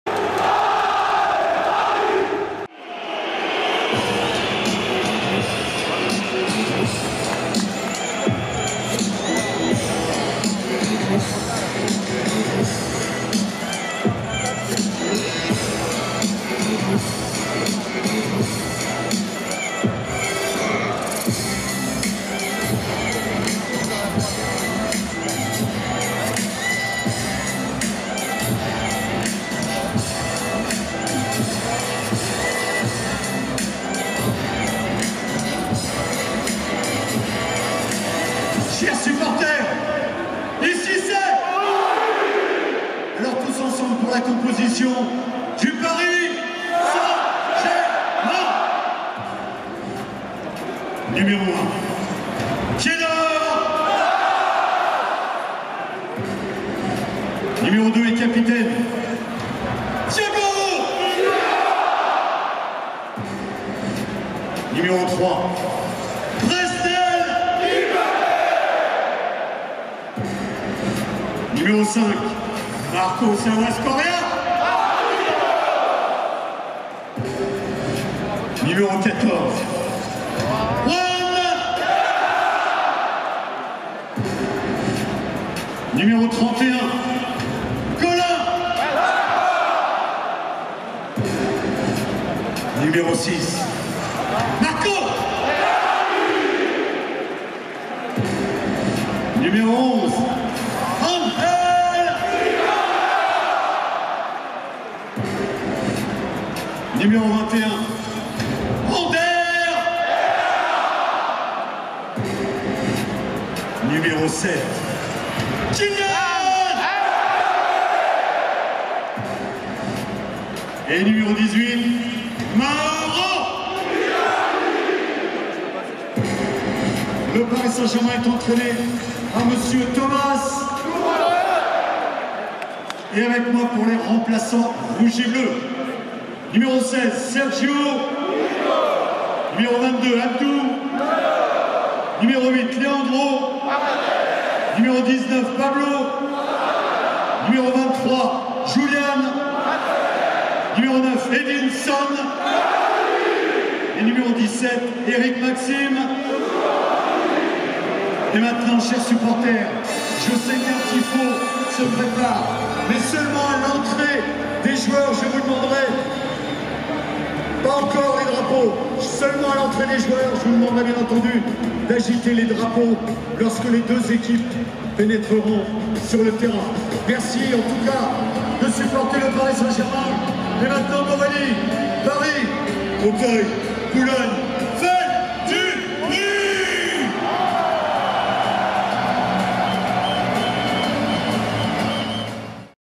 PSG / OM 27.10.2019 : 4-0 (L1 J11) 3/6 : Composition du PSG
PSG / OM - Paris - Parc des Princes
Ligue 1 - 11ème journée - Dimanche 27 octobre 2019
- Annonce de la composition du PSG -